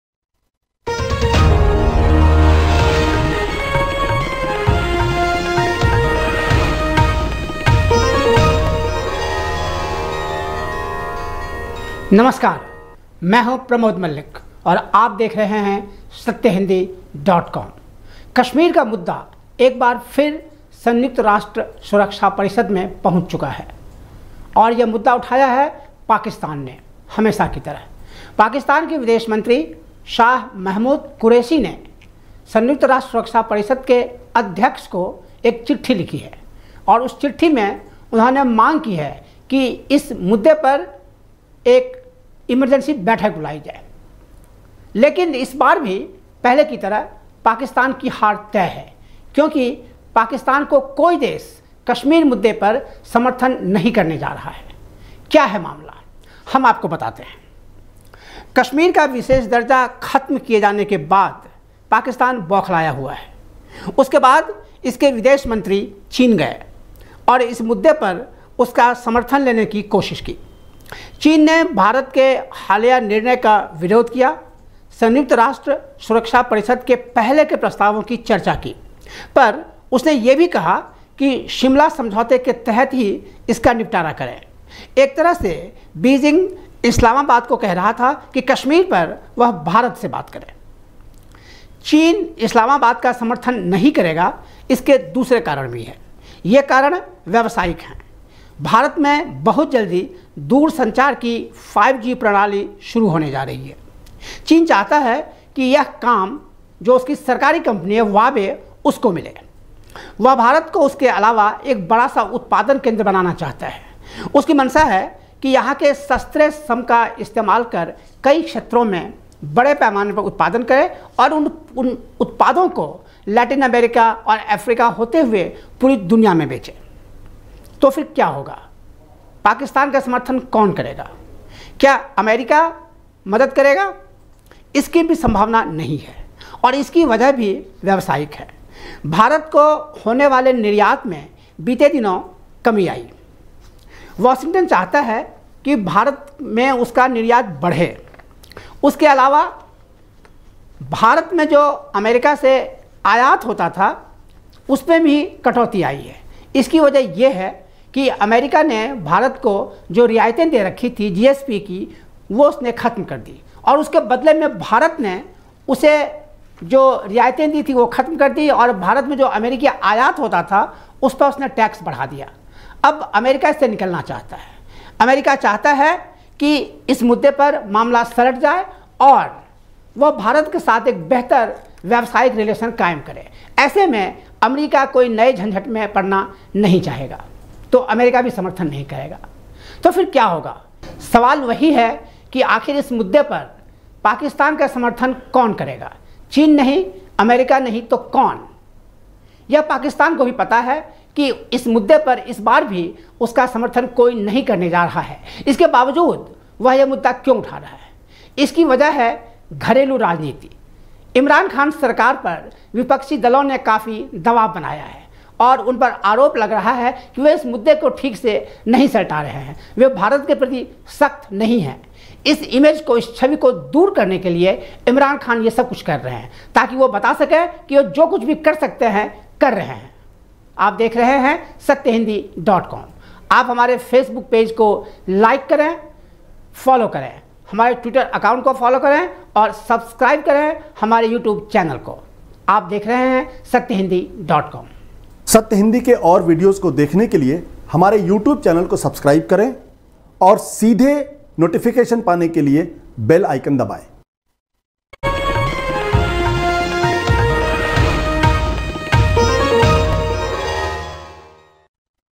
यूएन में कश्मीर : पाकिस्तान की हार तय?
पाकिस्तान ने कश्मीर मुद्दे पर संयुक्त राष्ट्र सुरक्षा परिषद की इमर्जेंसी बैठक की माँग की है। क्या वह इसमें कामयाब होगा? कोई देश उसका समर्थन  नहीं करने जा रहा है। सत्य हिन्दी के लिए प्रमोद मल्लिक का विश्लेषण।